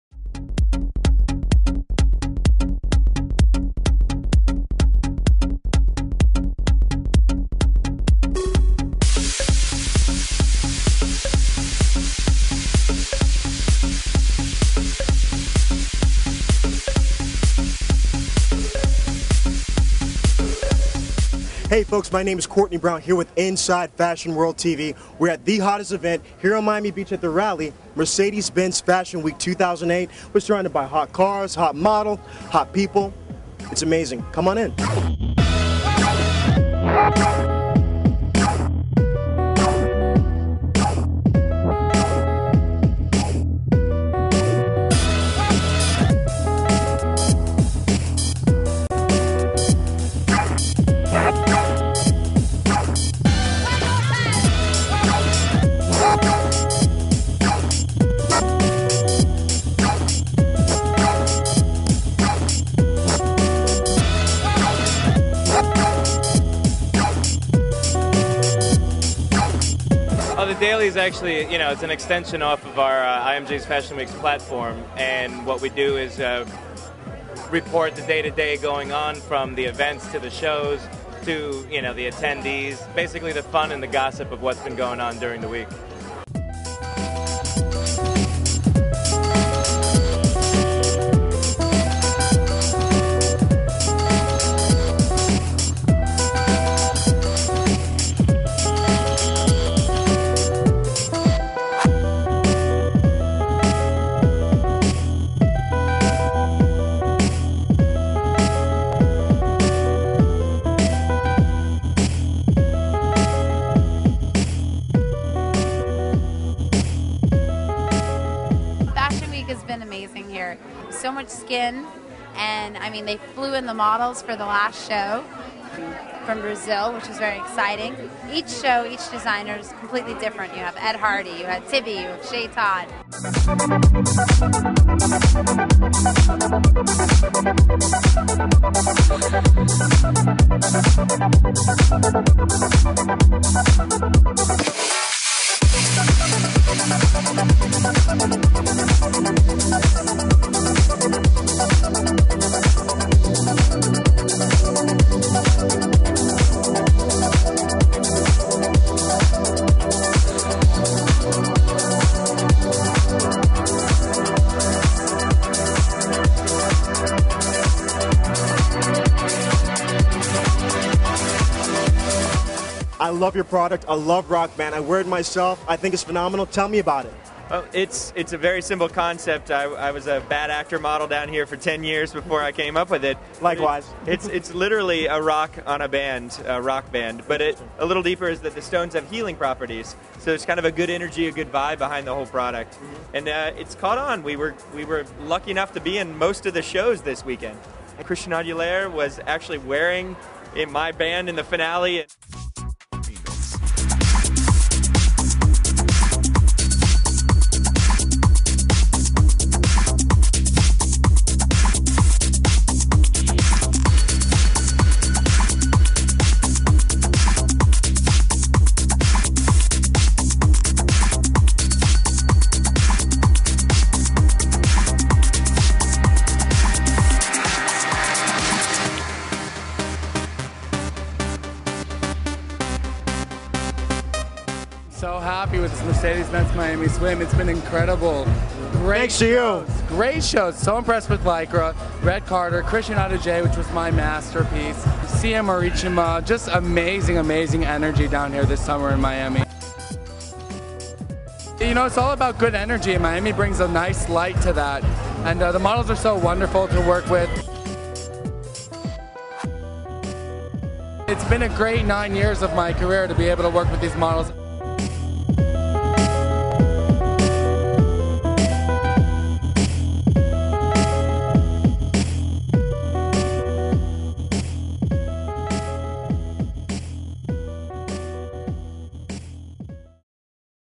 Mercedes Benz Swim Week Miami 09 Collection
Kourtney 'with a K' Brown Hosts Mercedes Benz Fashion Week: interviews